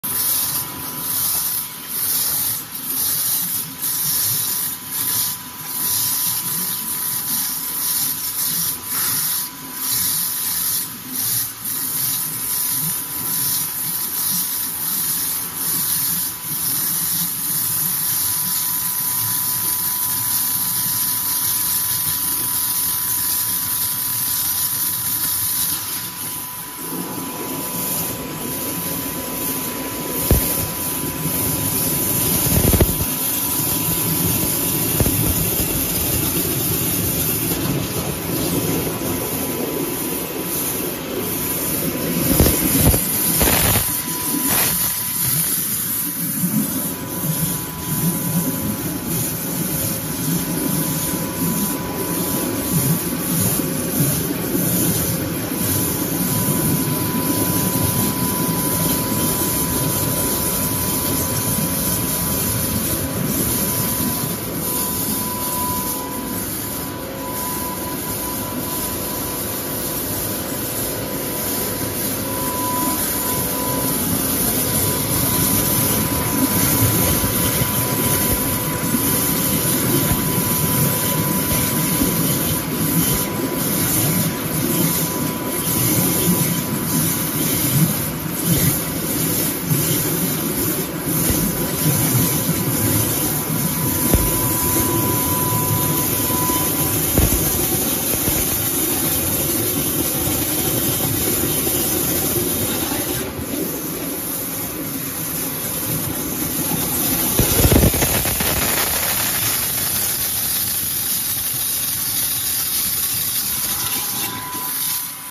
2005 Flow IFB 6024 Waterjet Cutting System
2005 Flow IFB 6024 Waterjet Cutting System (#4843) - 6’ X 24’ Waterjet Cutting System with Flow PC Controls (Upgraded in 2018) and Rebuilt Intensifier Pump.